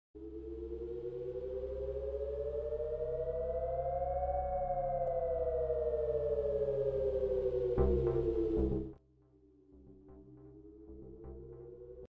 Space animation: 2D digital design
This is a project that I worked on to learn the motion path and armature skills in Adobe Animate.